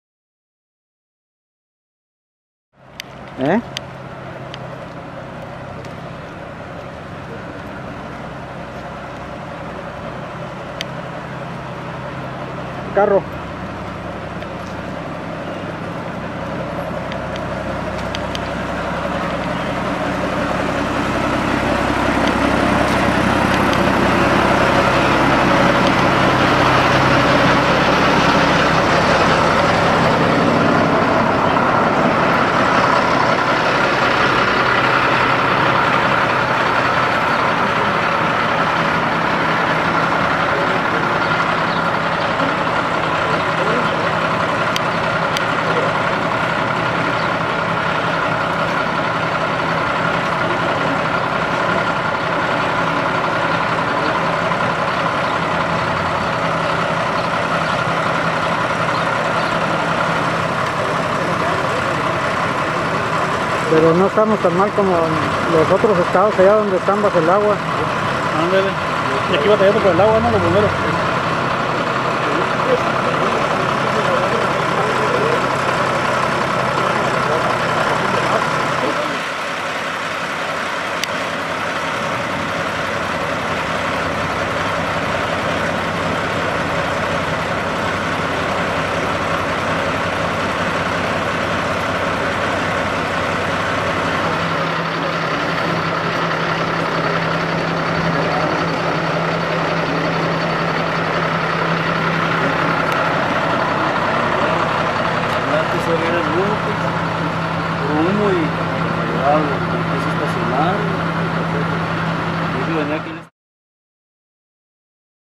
Se incendia pick up
Una falla en el sistema de abastecimiento de combustible provocó que un pick up Toyota con placas de circulación de California 4KNY22 sufriera un incendio en el motor, que requirió de la ayuda del cuerpo de bomberos de Ensenada para sofocar las llamas que prácticamente quemaron el motor.